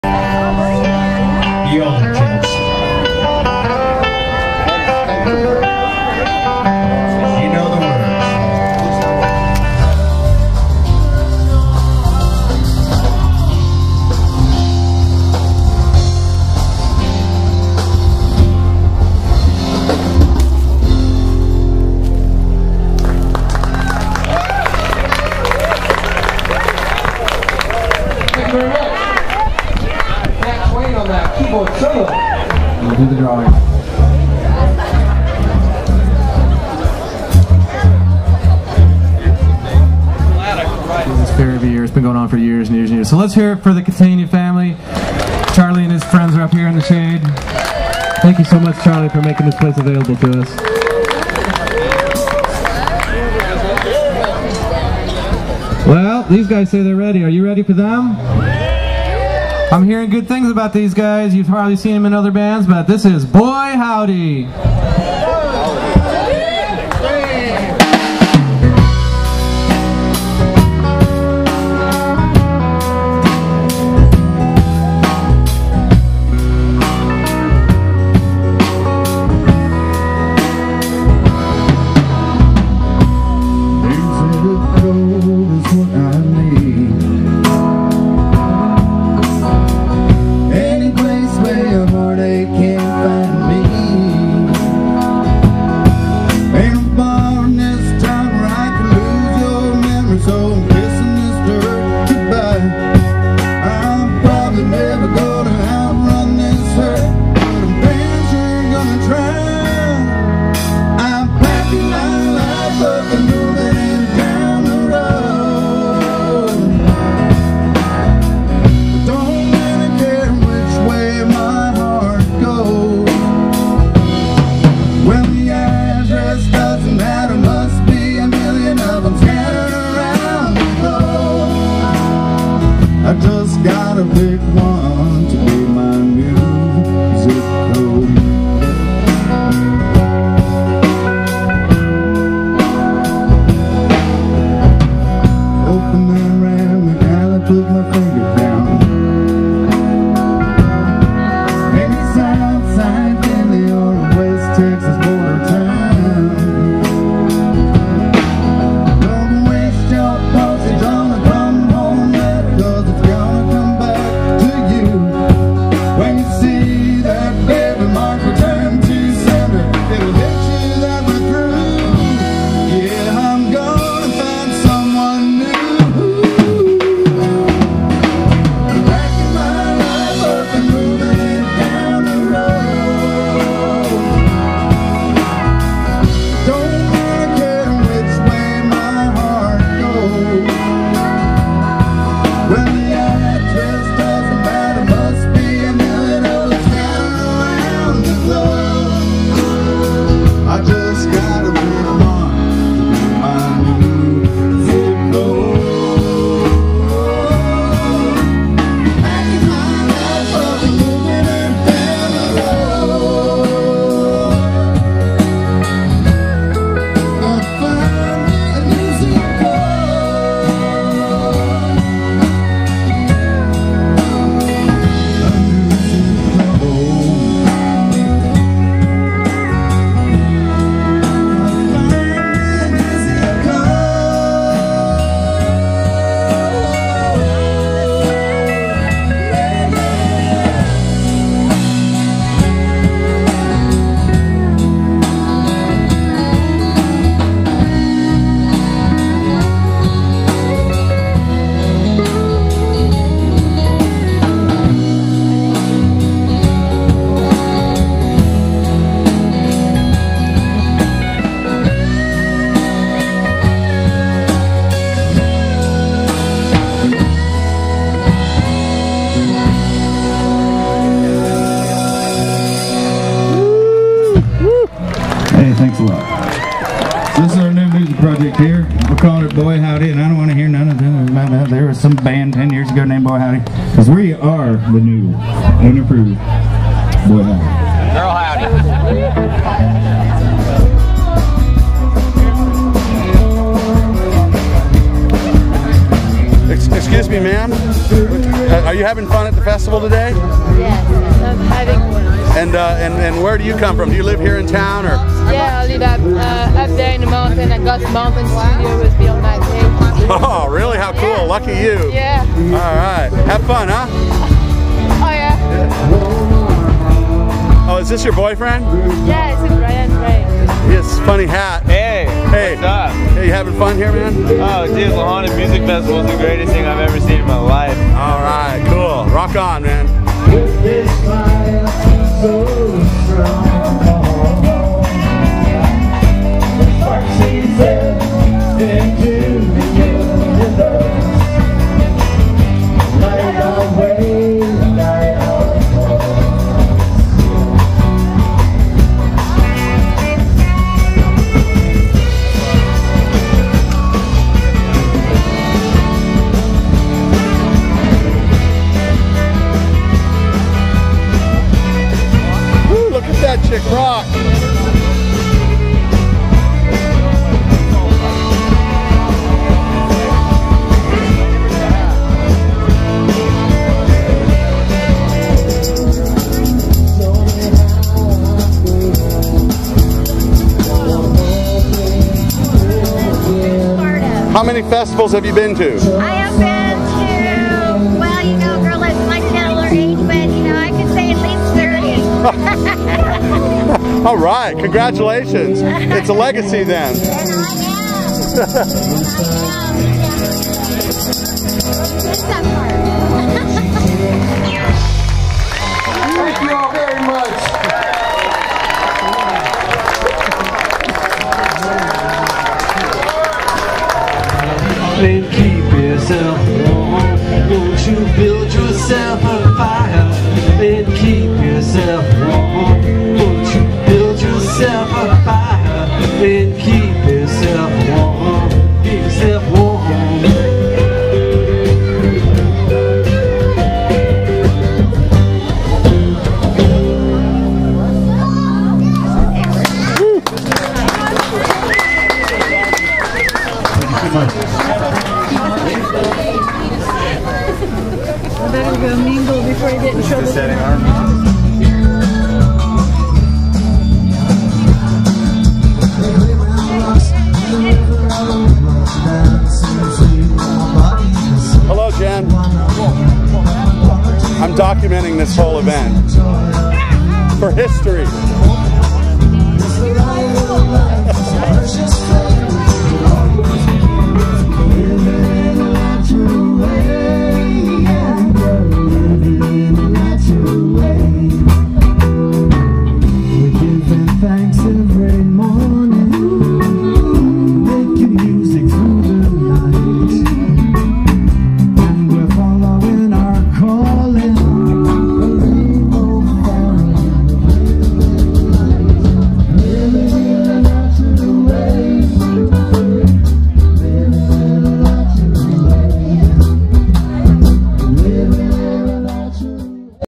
2010 La Honda Music Festival
One of the great music events that have made the Santa Cruz Mountains of California the stuff of legends in rock history. The La Honda Music Festival has been happening for many decades. In the film, the Tambourine Lady claims to have been to over 30 festivals! The sixties have not been forgotten by many of the attendees, although the music leans more classic country than acid rock. The band "Boy Howdy" was a festival highlight featuring a female singer who lives on a horse ranch! Video by W.J. McKay of gothmountain.com.